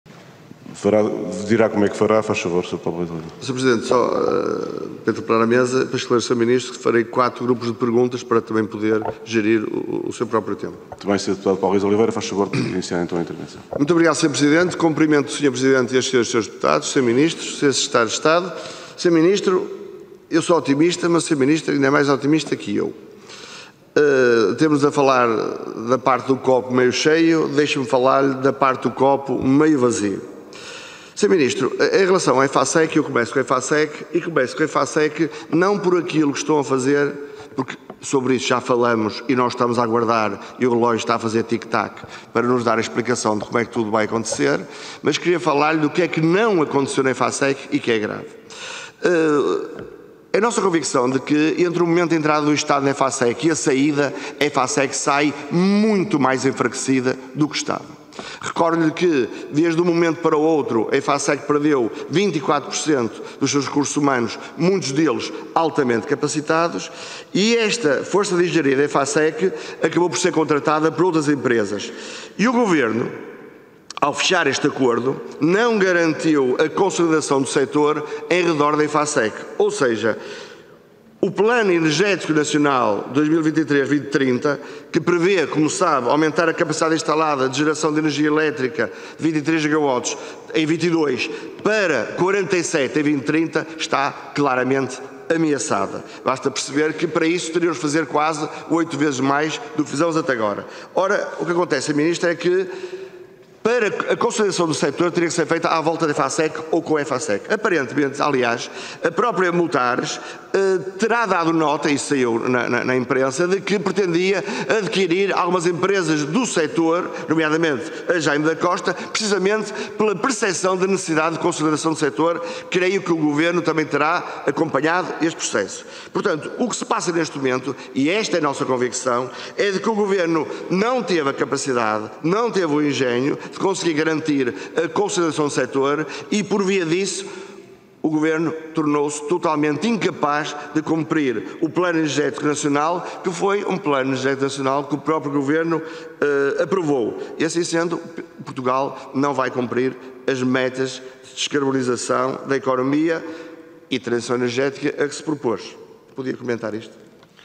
Intervenção de Paulo Rios de Oliveira na Comissão de Economia
Comissão de Economia, Obras Públicas, Planeamento e Habitação
Audição do Ministro da Economia e do Mar. 05-07